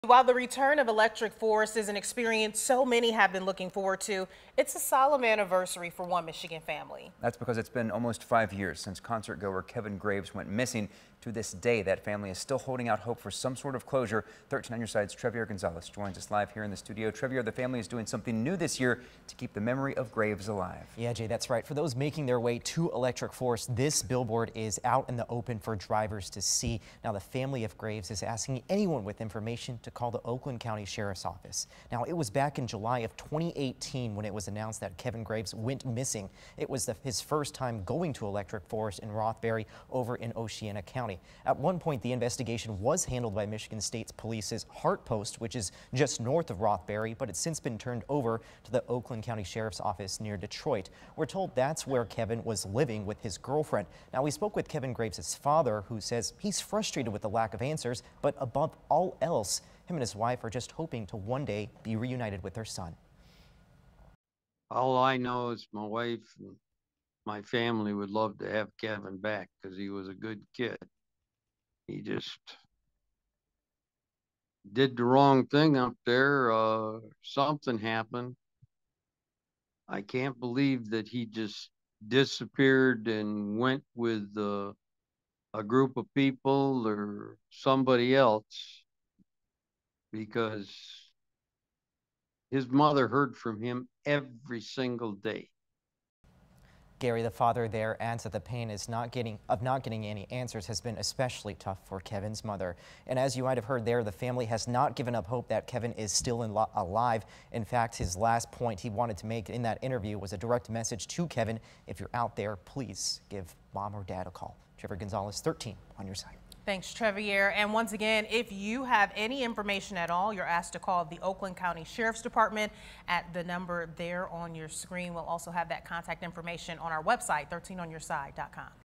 Family of man who disappeared at Electric Forest still looking for answers
While the return of Electric Forest is an experience so many have been looking forward to, it's a solemn anniversary for one Michigan family.

It's been almost five years since concertgoer Kevin Graves went missing.

To this day, his family is still holding out hope for some sort of closure in his disappearance.